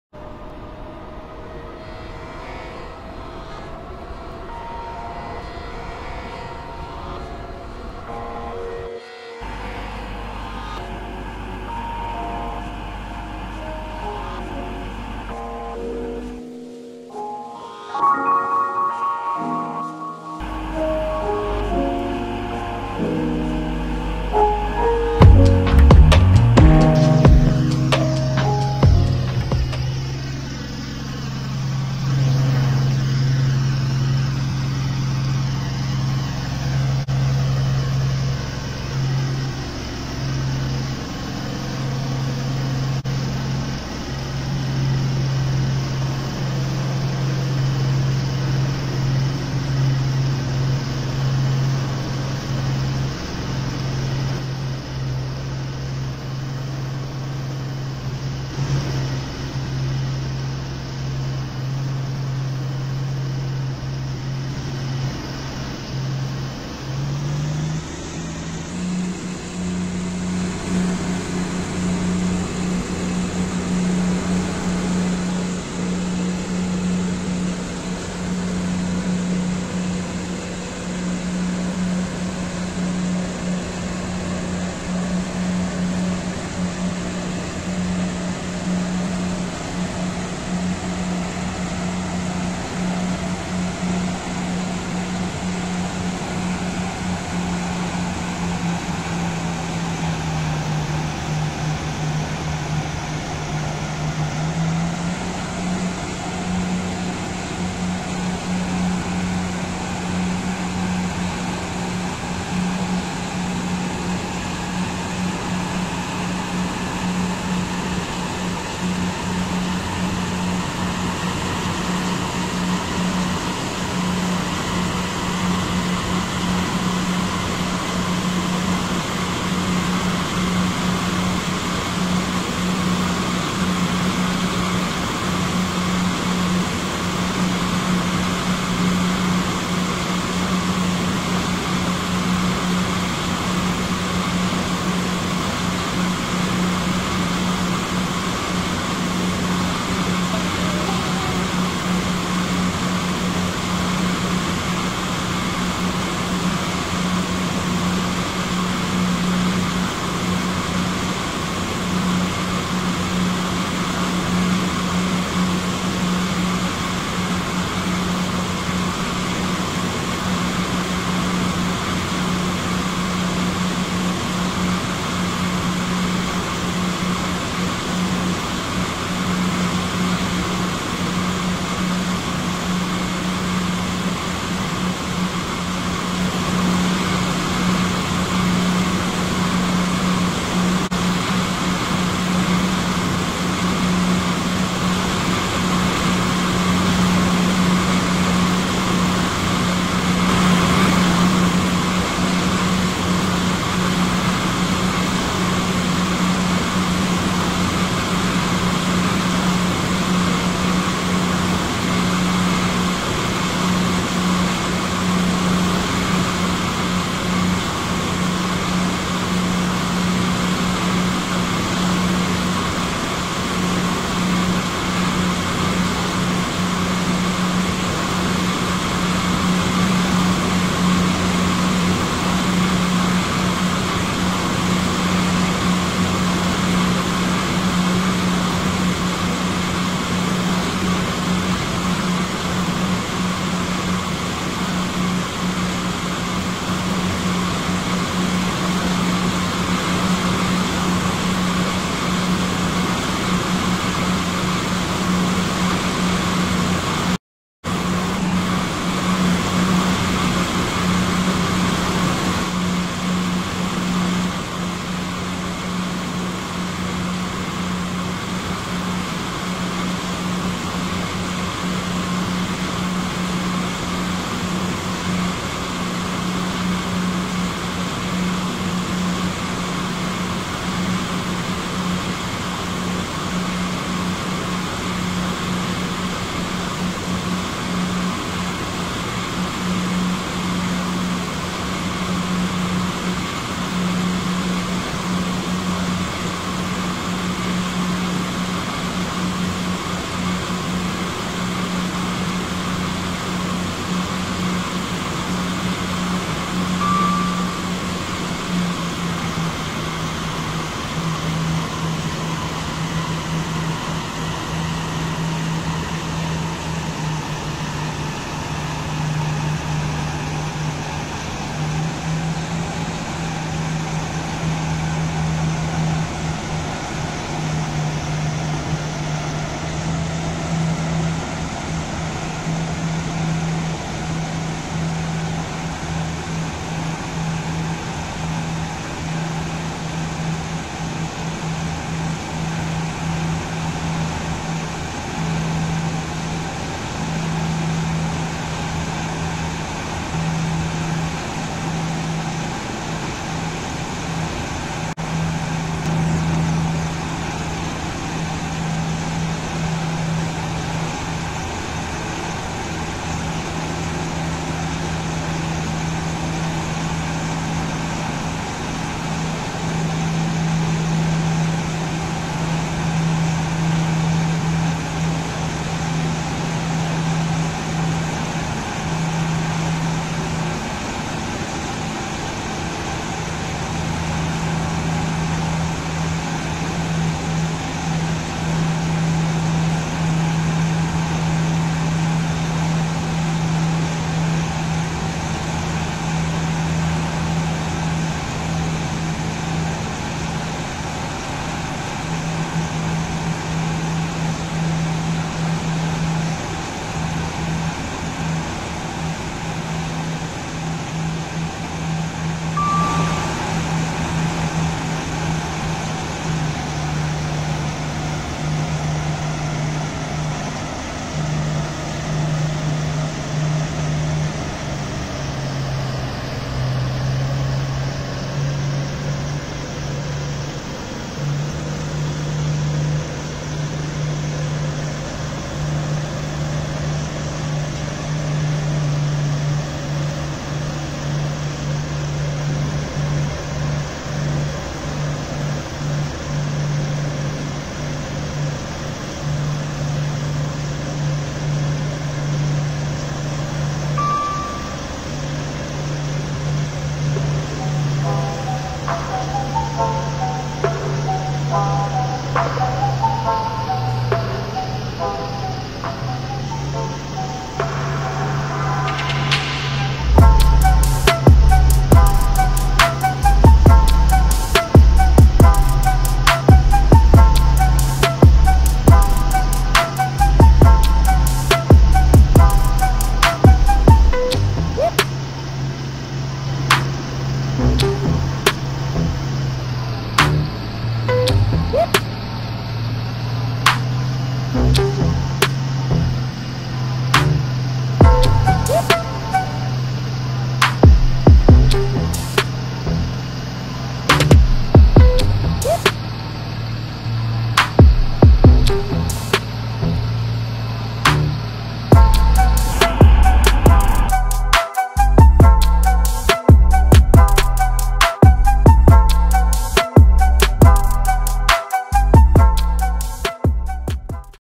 Class 220 Voyager {Mind the Gap}{Bentley - LC Isembard}
Well this video is another ep of A class 220 But Between Bentley and Lonchester Isembard.